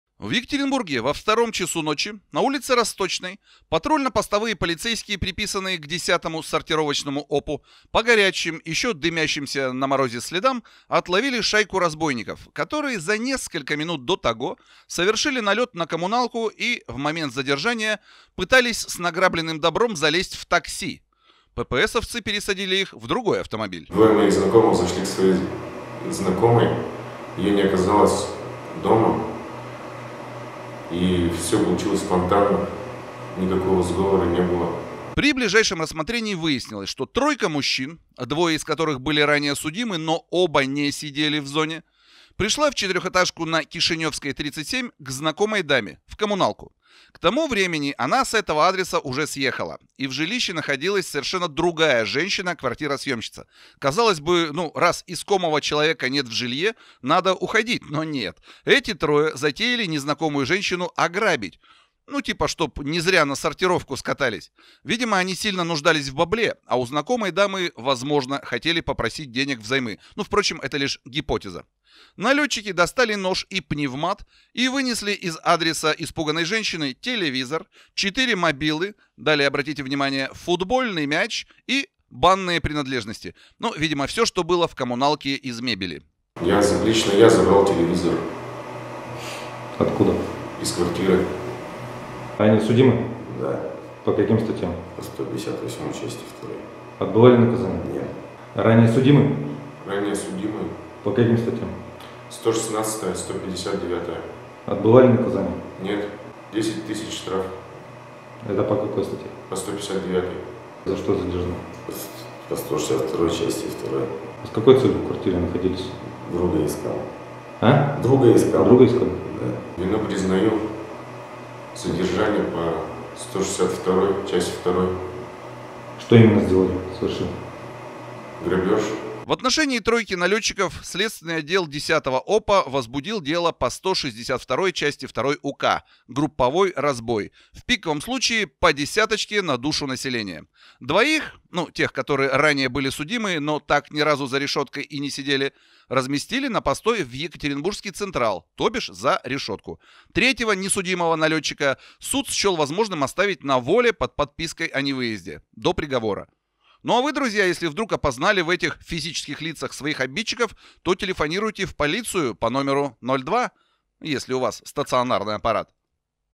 Налётчики вынесли из адреса телевизор, мобилы и футбольный мяч
Хлопнули разбойников при посадке в такси